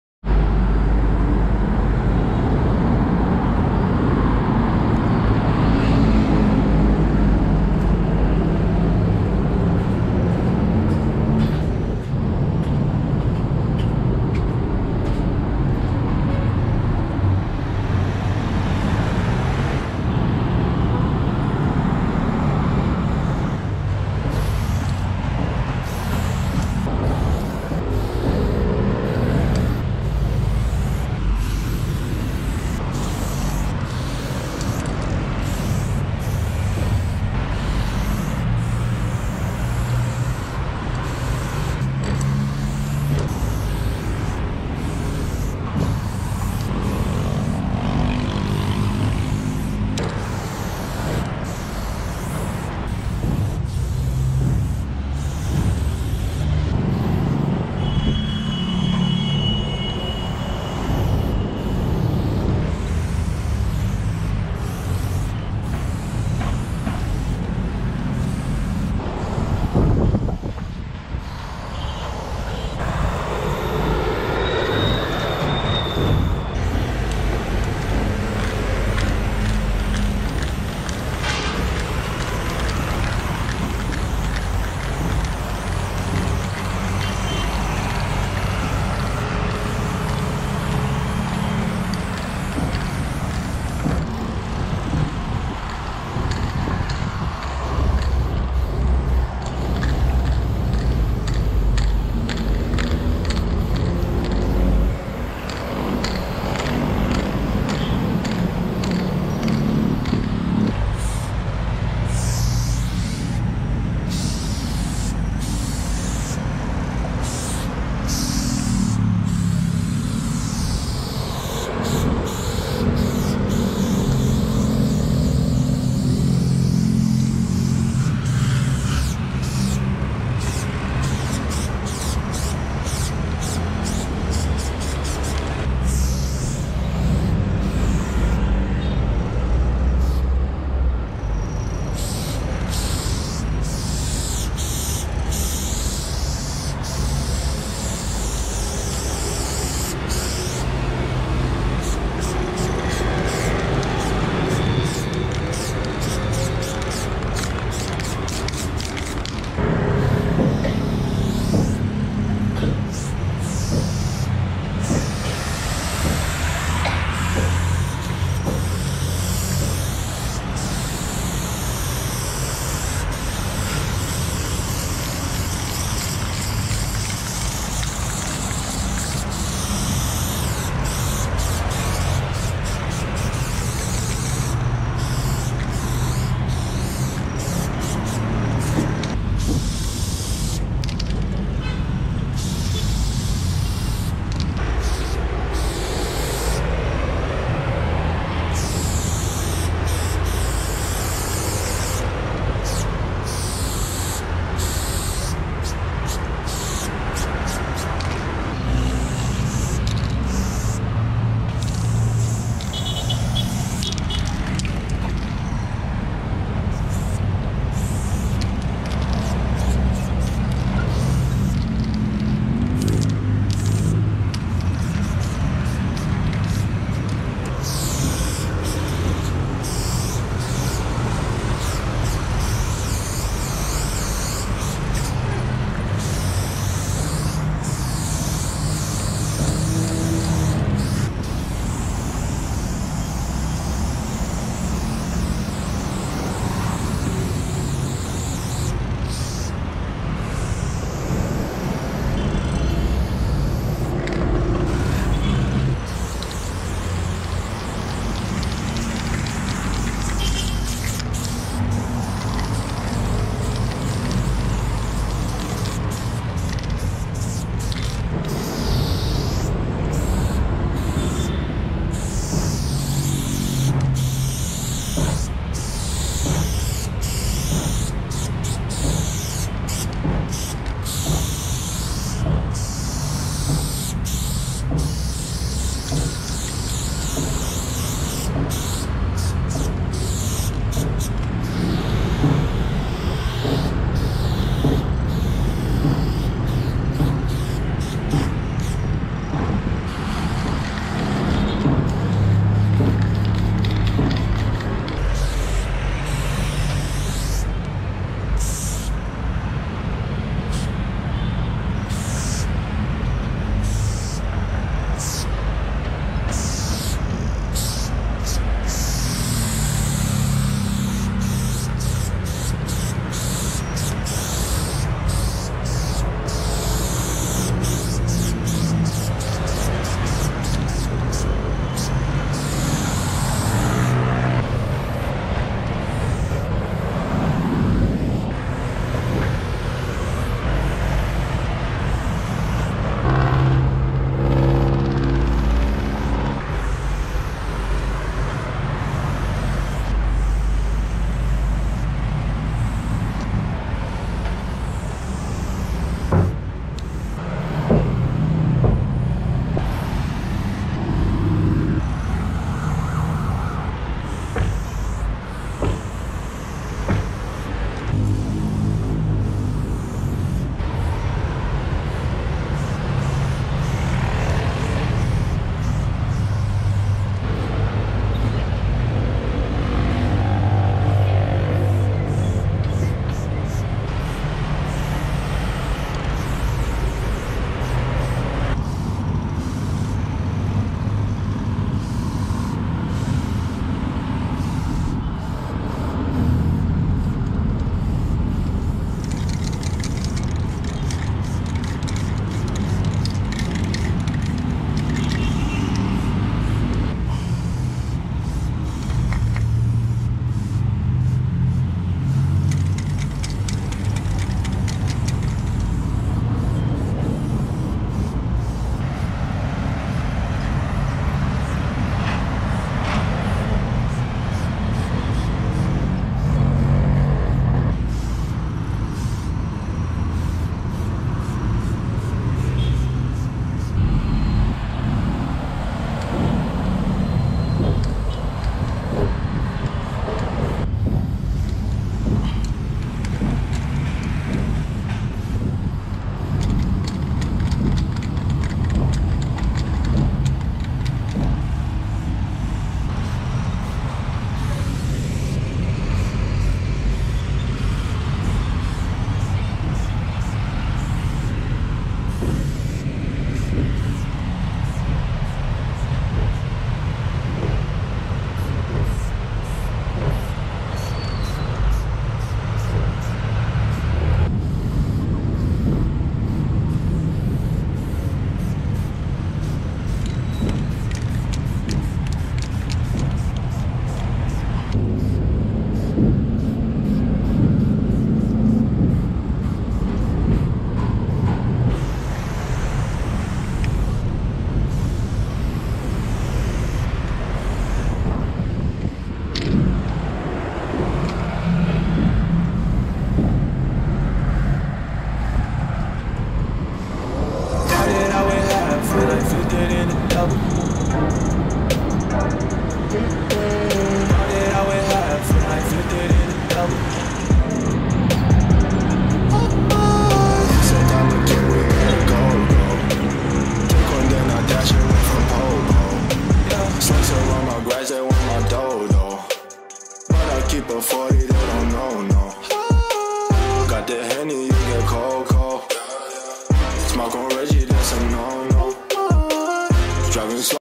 The Secret Writer 4 - YouTubes Biggest Graffiti Exchange
I use OneTake Graffiti Cans! I recommend Trying it out.

Round 4 features walls by:
Saturday
(Magnes, Suberebus, Idem, Normie, Feron, Repso, Code5, Eks, Fusi, Dems.)
Sunday
(Aize, Daiso, Kash, Nevs, Rust, Profets, Mauy, Slane, Core, Goke.)